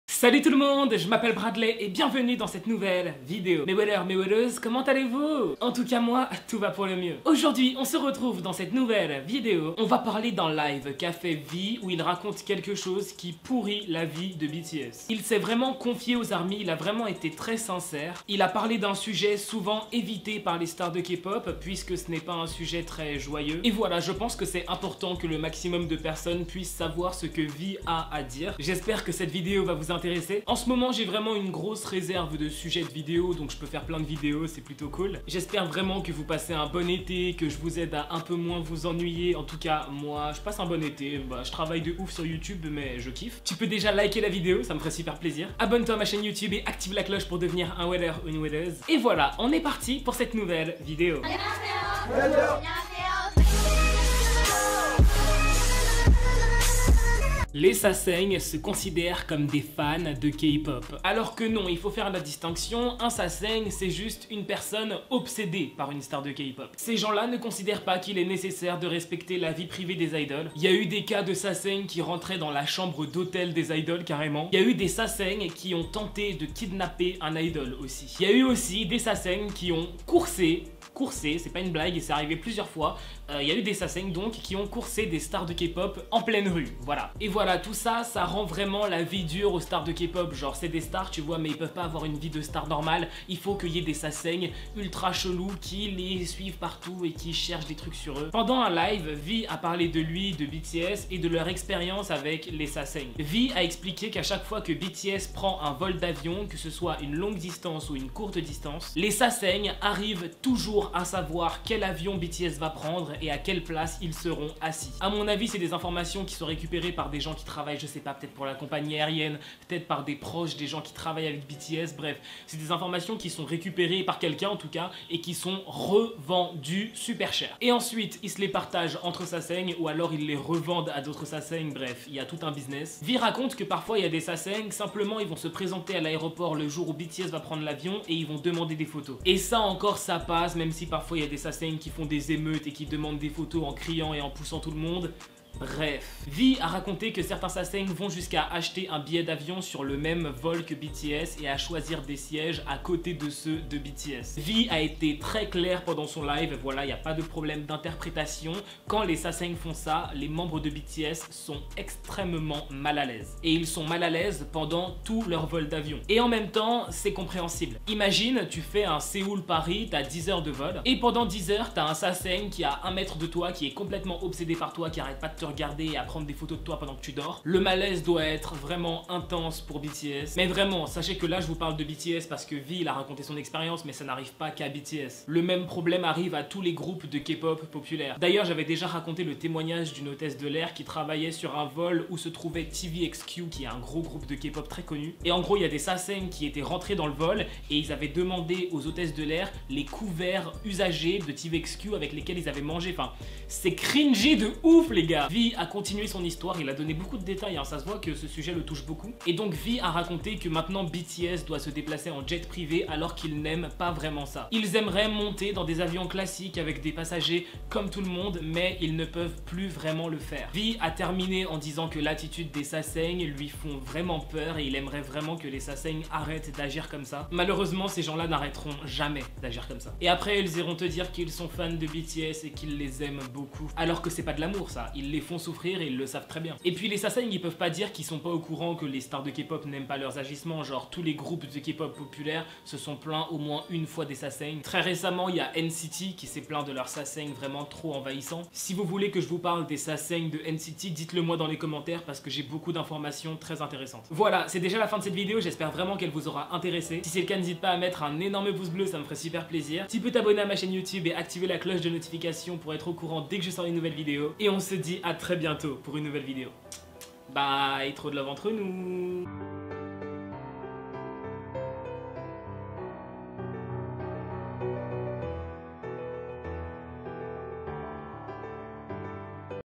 V PARLE DES FANS SASAENGS QU'IL DÉTESTE !
nouvelle vidéo sur V de BTS qui a parlé des fans sasaengs qui embête BTS

►Questions fréquemment posées
❤️Âge: 17 ans
❤️Caméra: Canon 600D
❤️Groupe préférer: BTS/BIGBANG

❤️ bradleywell